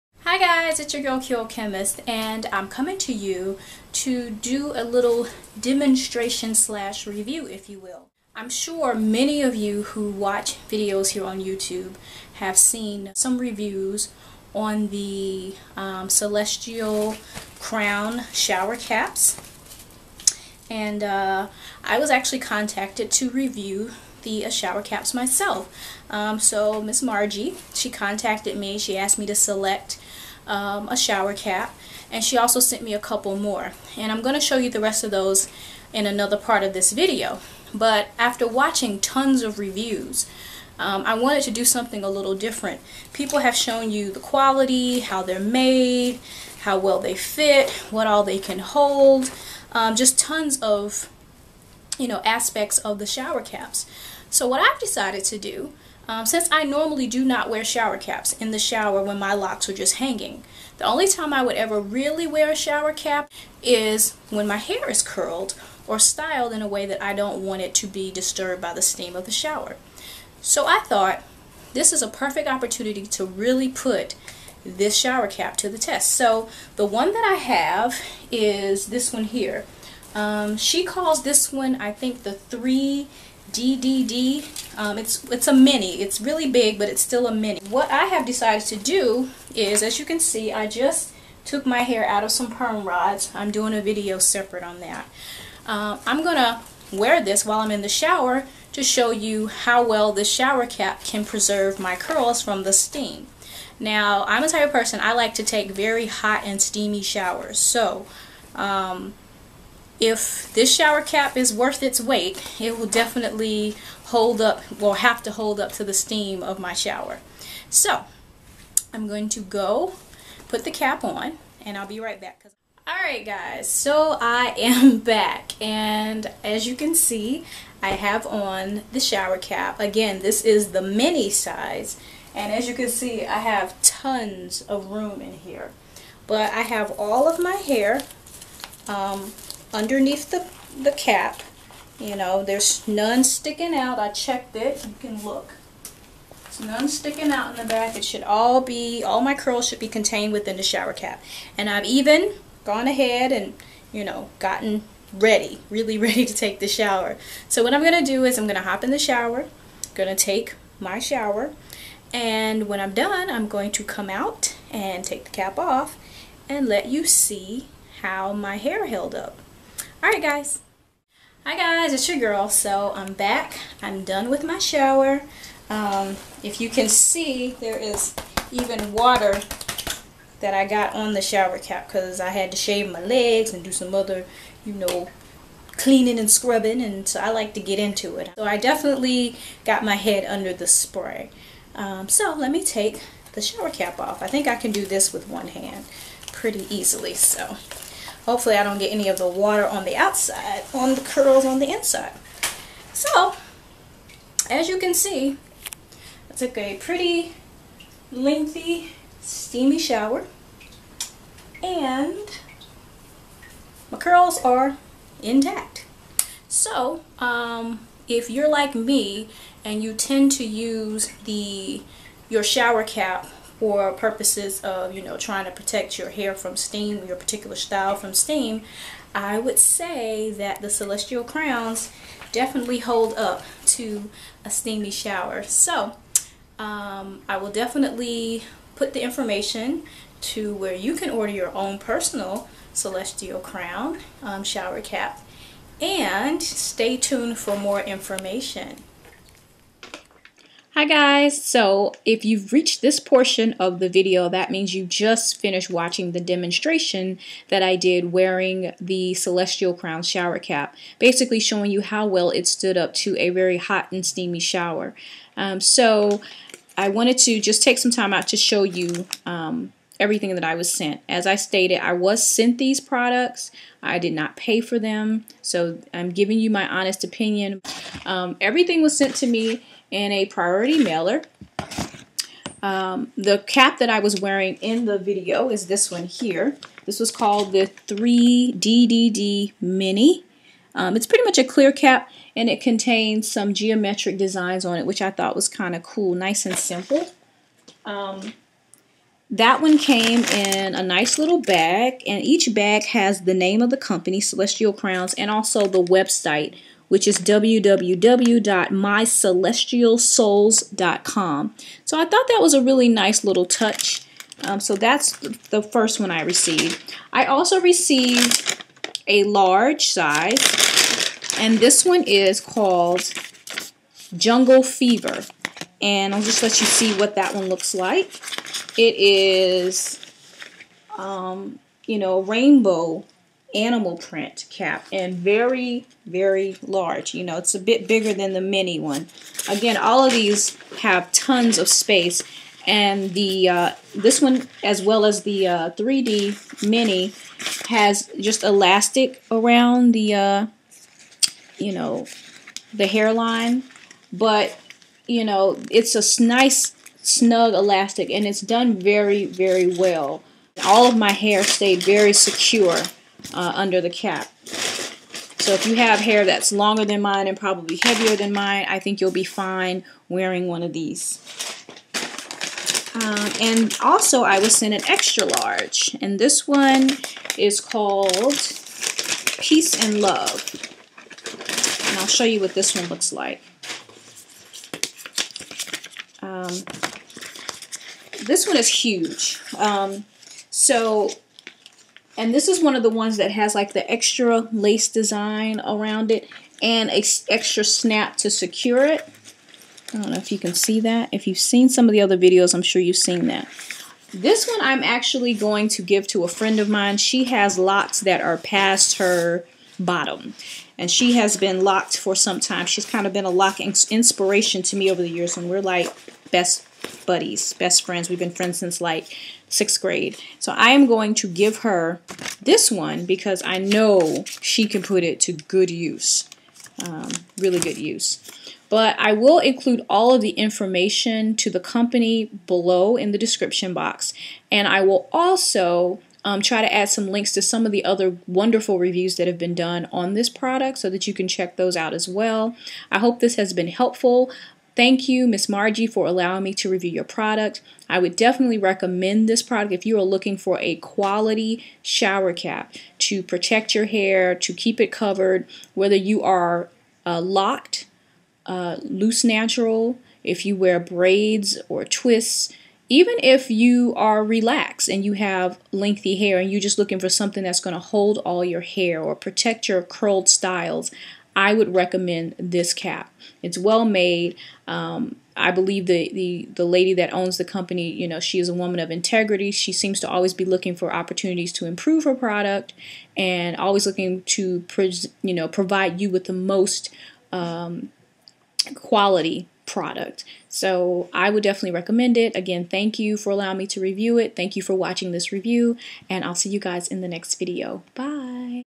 Product Review: Celestial Crowns Shower Caps (Demo and Review)
to find your perfect crown